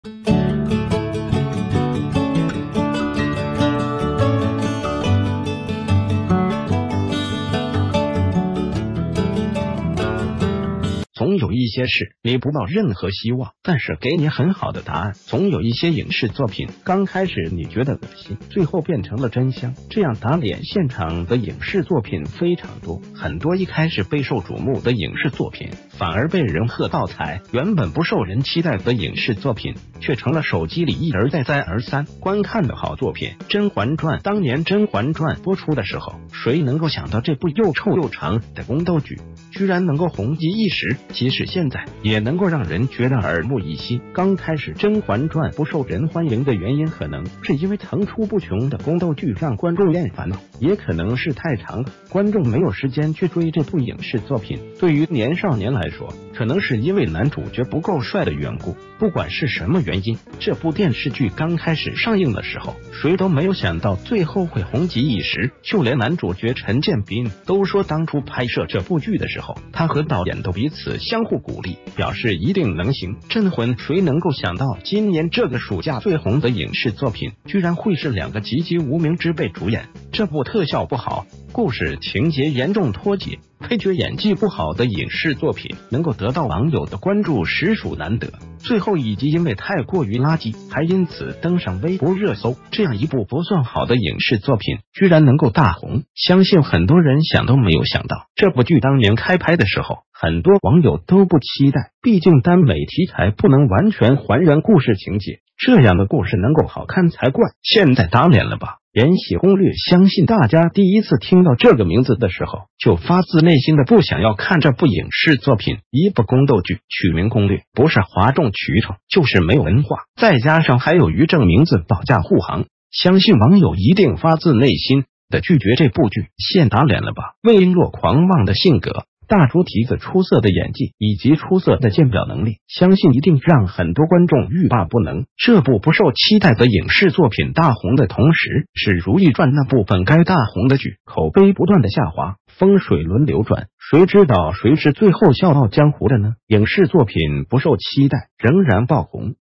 “本來不看好，結果跪舔”的電視劇，網友：大型打臉現場
總有一些事，你不抱任何希望，但是給你很好的答案，總有一些影視作品，剛開始你覺得惡心，最后變成了真香，這樣打臉現場的影視作品非常多，很多一開始備受矚目的影視作品，反而被人喝倒彩，原本不受人期待的影視作品，卻成了手機里一而再再而三觀看的好作品。
    《甄嬛傳》
    當年甄嬛傳播出的時候，誰能夠想到這部又臭又長的宮斗劇居然能夠紅極一時，即使現在，也能夠讓人覺得耳目一新。剛開始甄嬛傳不受人歡迎的原因可能是因為層出不窮的宮斗劇讓觀眾厭煩了，也可能是太長了，觀眾沒有時間去追這部影視作品，對于年少年來說，可能是因為男主角不夠帥的緣故，不管是什么原因，這部電視劇剛開始上映的時候，誰都沒有想到最后會紅極一時，就連男主角陳建斌都說當初拍攝這部劇的時候，他和導演都彼此相互鼓勵，表示一定能行。
    《鎮魂》
    誰能夠想到今年這個暑假最紅的影視作品，居然會是兩個籍籍無名之輩主演？這部特效不好、故事情節嚴重脫節、配角演技不好的影視作品，能夠得到網友的關注實屬難得，最后一集因為太過于垃圾，還因此登上微博熱搜，這樣一部不算好的影視作品，居然能夠大紅，相信很多人想都沒有想到。這部劇當年開拍的時候，很多網友都不期待，畢竟耽美題材不能完全還原故事情節，這樣的故事能夠好看才怪，現在打臉了吧。
    《延禧攻略》
    相信大家第一次聽到這個名字的時候，就發自內心地不想要看這部影視作品。一部宮斗劇，取名攻略，不是嘩眾取寵，就是沒有文化，再加上還有于正名字“保駕護航”，相信網友一定發自內心地拒絕這部劇，現打臉了吧？魏瓔珞狂妄的性格，大豬蹄子出色的演技，以及出色的鑒婊能力，相信一定讓很多觀眾欲罷不能。這部不受期待的影視作品大紅的同時，是《如懿傳》那部本該大紅的劇，口碑不斷地下滑，風水輪流轉，誰知道誰是最后笑傲江湖的呢？
    影視作品不受期待，仍然爆紅，暗示大家，即使你覺得你已經身在低谷了，不能再下降的時候，你就會上升，船到橋頭自然直嘛。